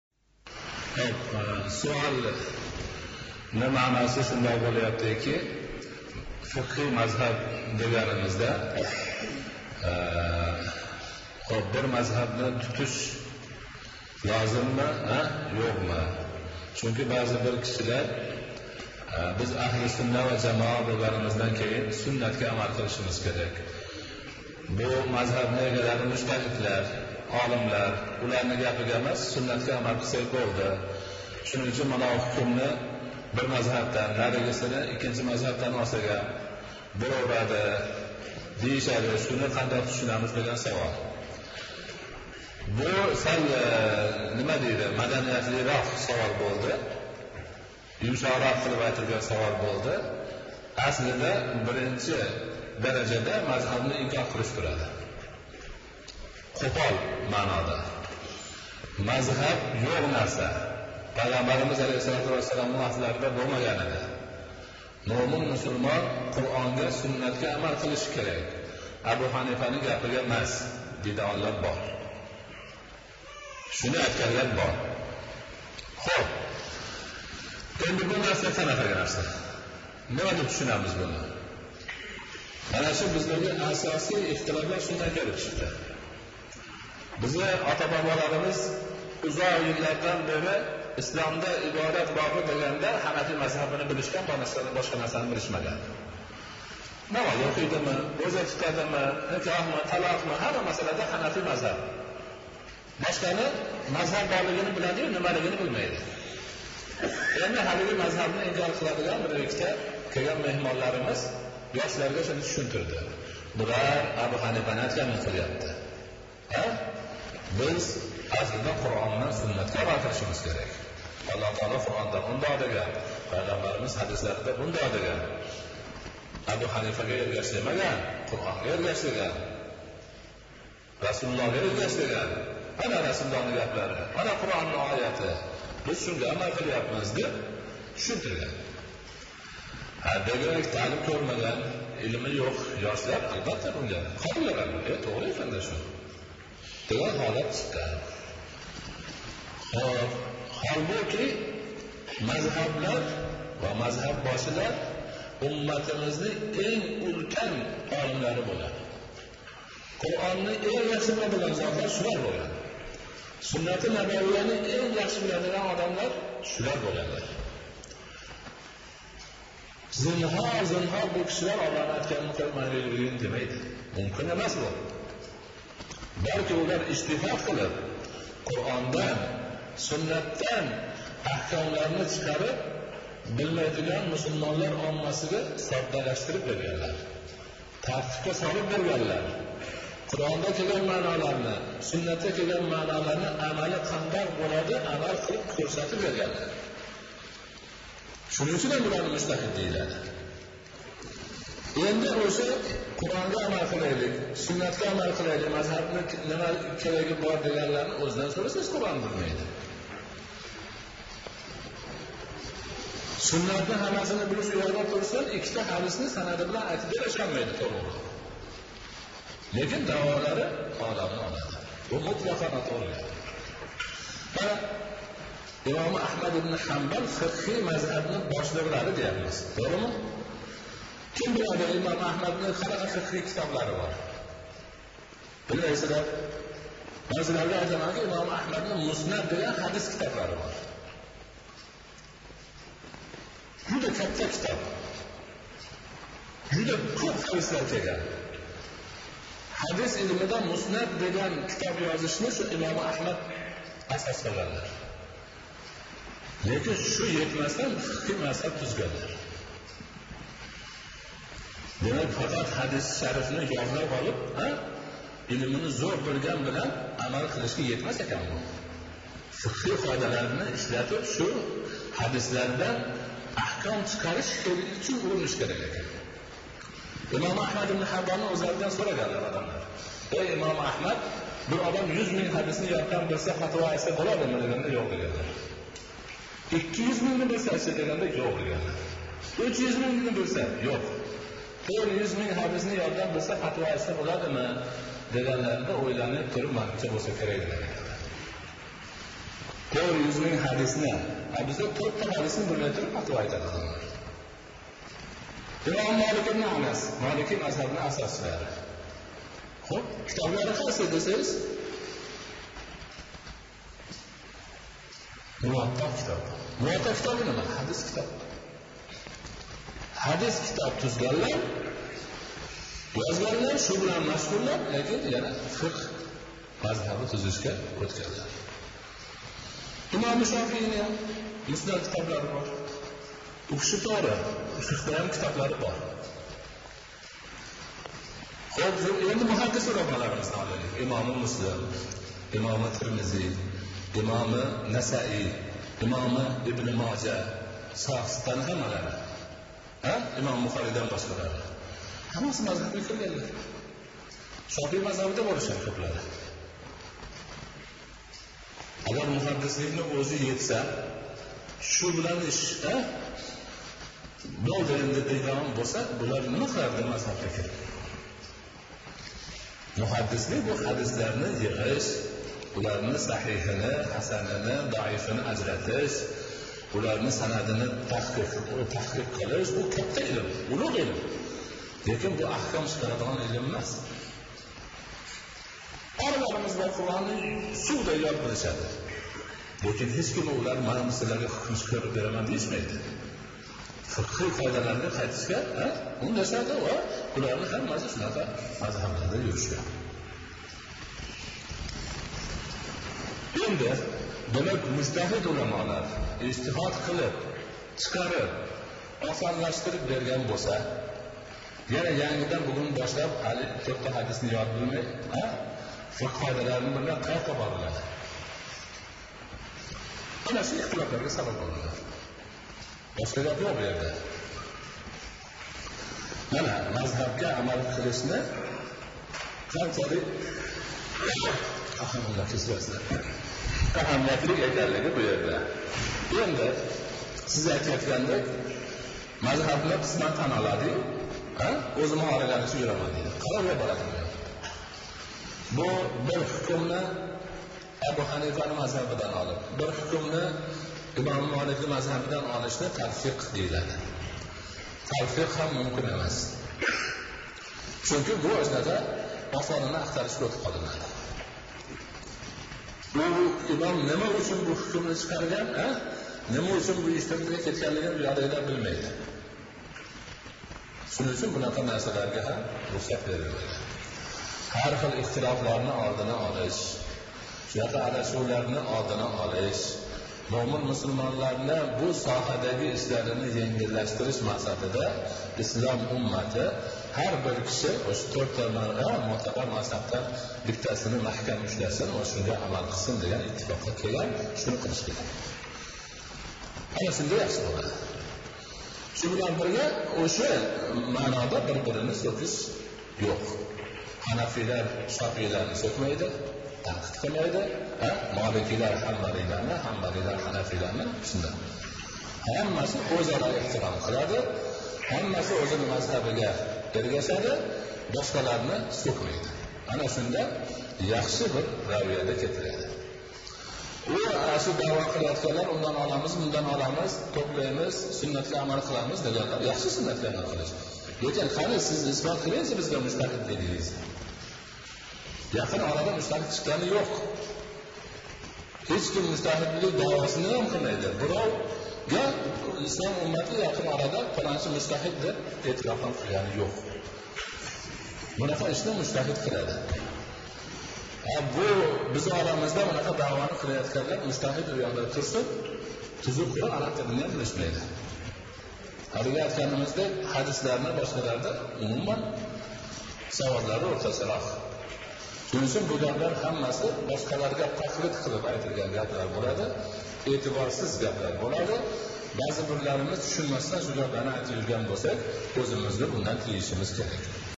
Мазҳаб ҳақида
Шайх Муҳаммад Содиқ Муҳаммад Юсуф